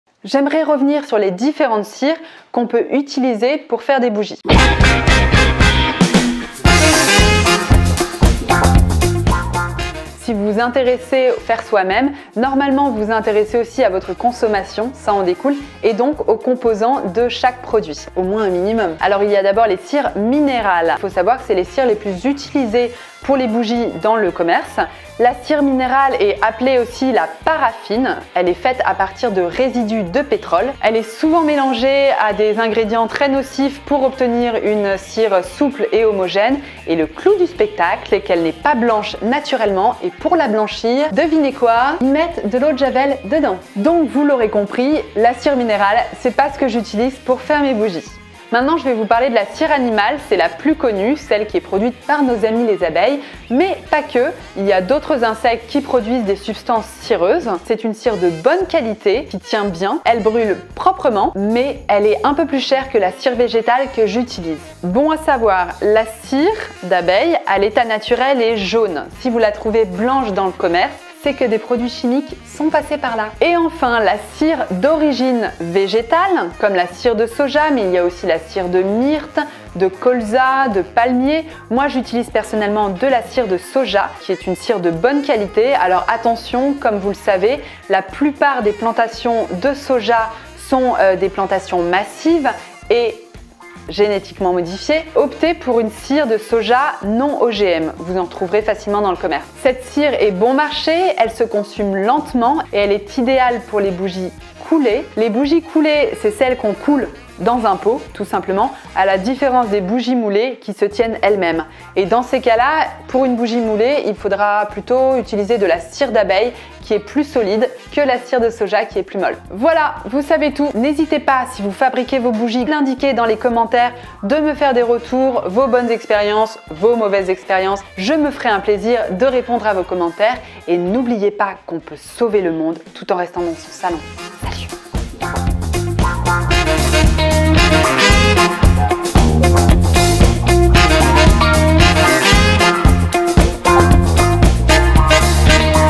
Pourquoi vous ne devez pas utiliser de cire minérale
Il y a trois types de cires pour fabriquer ses propres bougies : minérale, végétale et animale. Dans cette vidéo, je vous explique les avantages et les inconvénients de chacune de ces cires.

Pour donner un coup de pouce à ma chaîne en regardant une pub de 20 secondes, c'est

Réalisation et montage : Mathilde Gaudéchoux